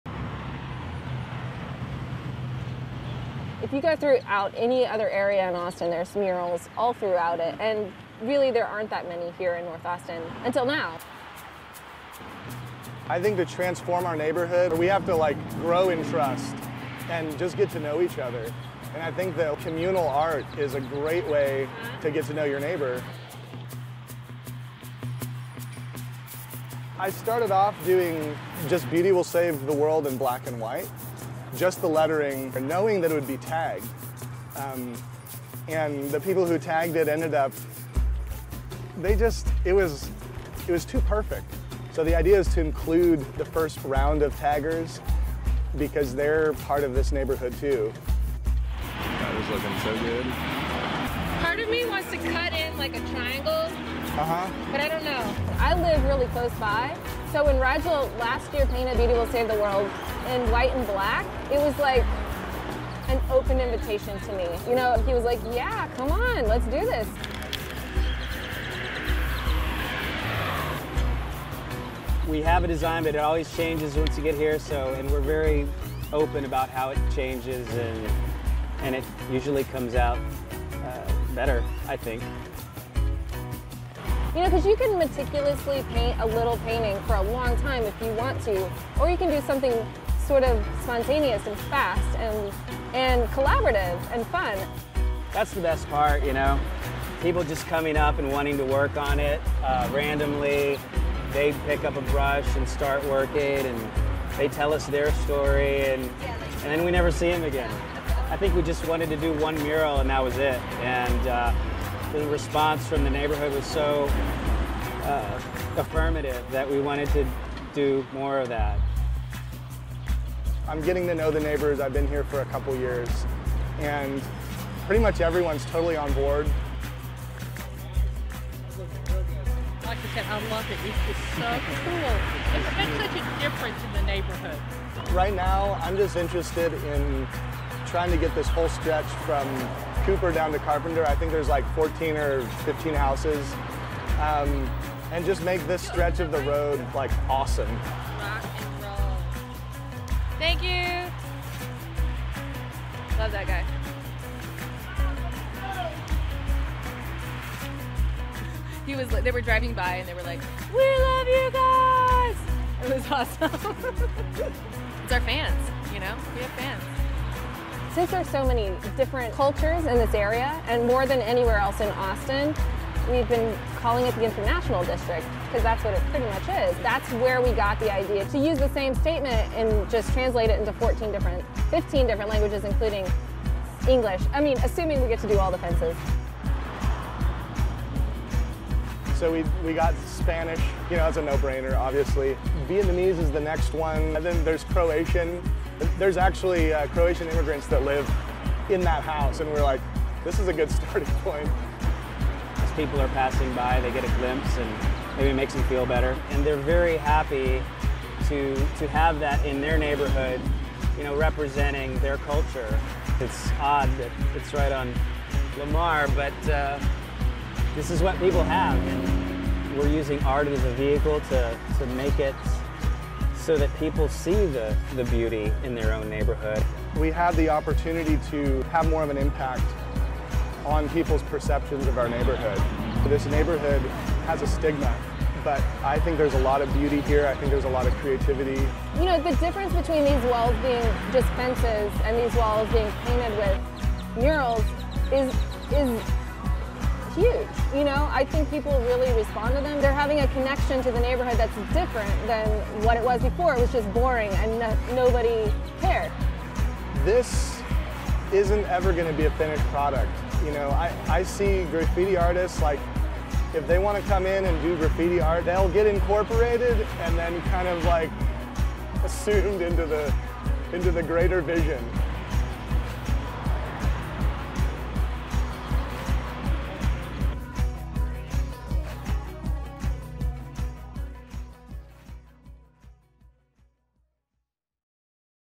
Beautiful Fences
In an effort to beautify their neighborhood and help build community spirit, artists and citizens in North Austin collaborate to paint a long row of formerly unassuming fences amidst the rush of traffic on Lamar Blvd. Repeatedly painting the phrase "beauty will save the world" in various languages, this diverse community demonstrates the power art has to change perceptions and increase pride in one's neighborhood across a variety of cultural backgrounds.